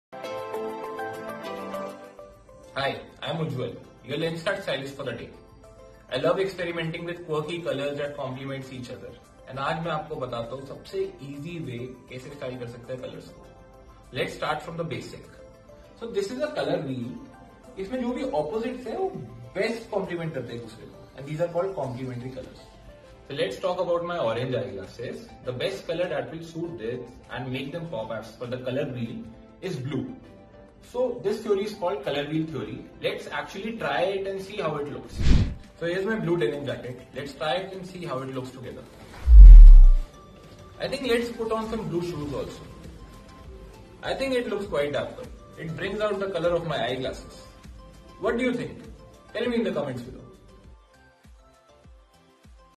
How To Pick Your Eyewear Choosing Colour Wheel Theory | Lenskart Stylist
Colour up in style! The LenskartStylist of the day is telling you everything you should know about the Colour Wheel Theory and using it to pick your perfect frames. Now flaunt your colourful, chic Lenskart frames with your outfit like a pro.